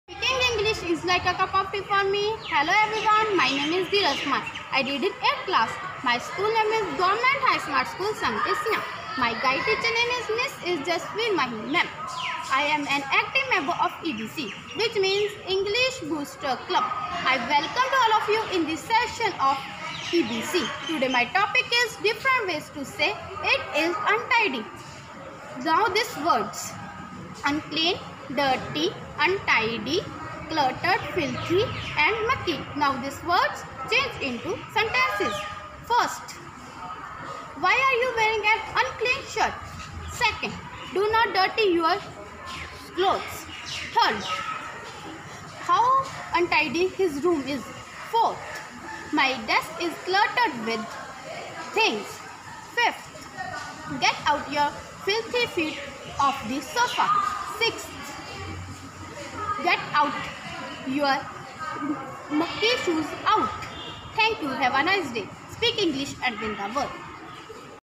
EBC, Dheeraj Kumar, class-8th, GHSS Sang Dhesian
EBC, Dheeraj Kumar, class-8th, GHSS Sang Dhesian, Topic-Different ways to say- it is untidy, Guide teacher-Madam JASBIR MAHI, School Incharge--Mr Kapil Dev, BM-Mr Avtar Lal, Goraya 1, Jalandhar